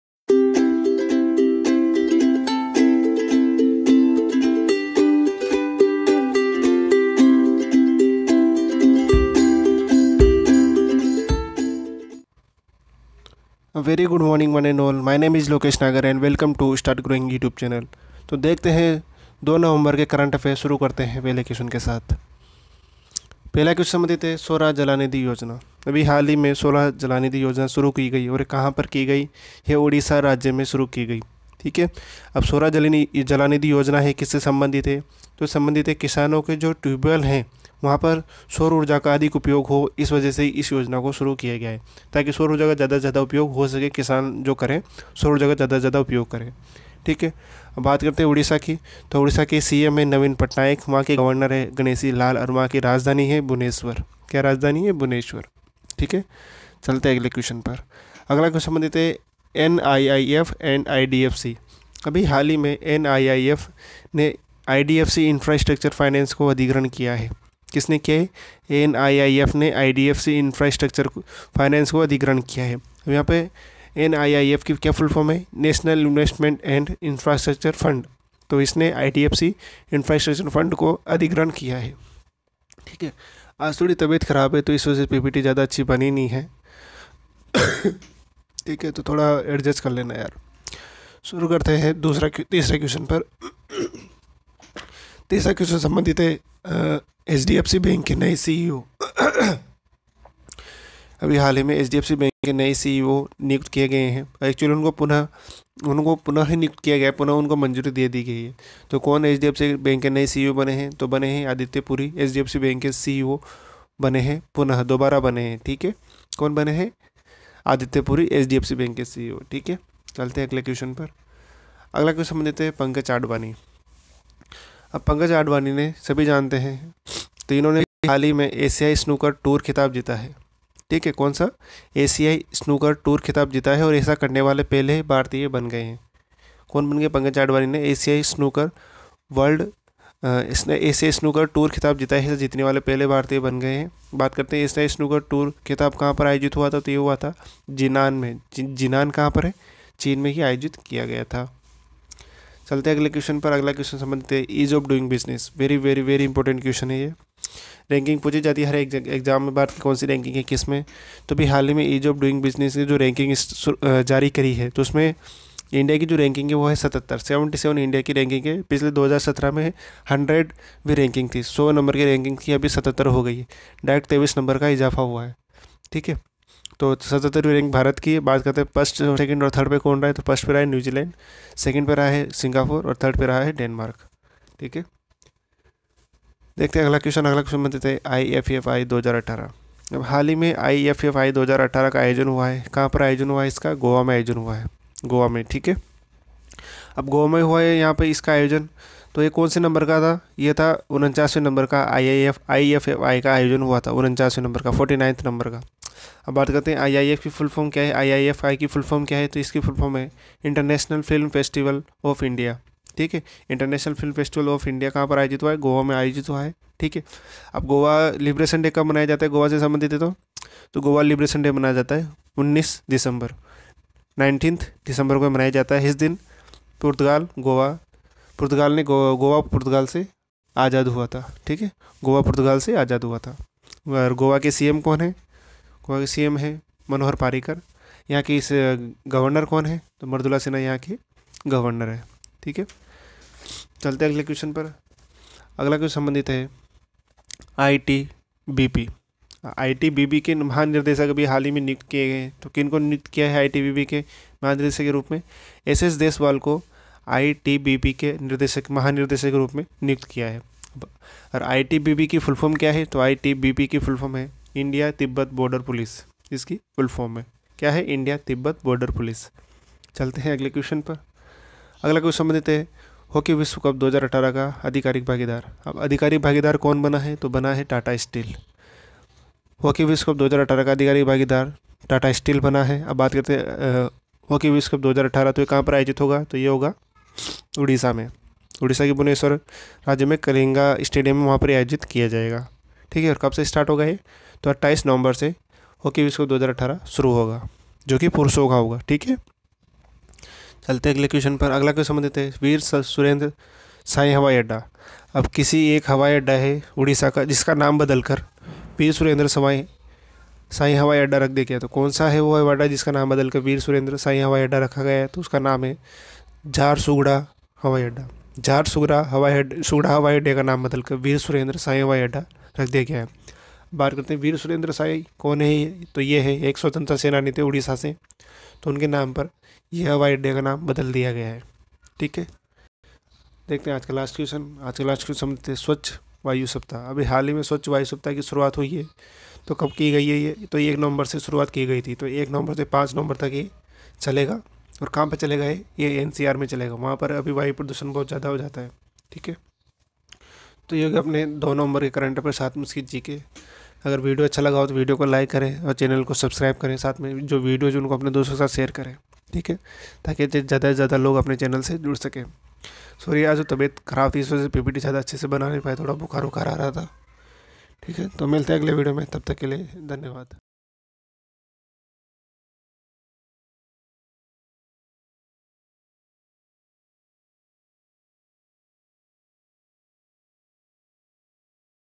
2 November 2018 important current affairs for competitive exams like SSC Railway Bank exams.
This video contains a complete current affairs of 2 November 2018 for those who are planning or preparing for any competitive exams like SSC Railway Bank and other states level exams as well , in this video I covered current affairs in order a proper manner with gernal knowledge related to each current affairs which will be very helpful for everyone who are really preparing for any exams.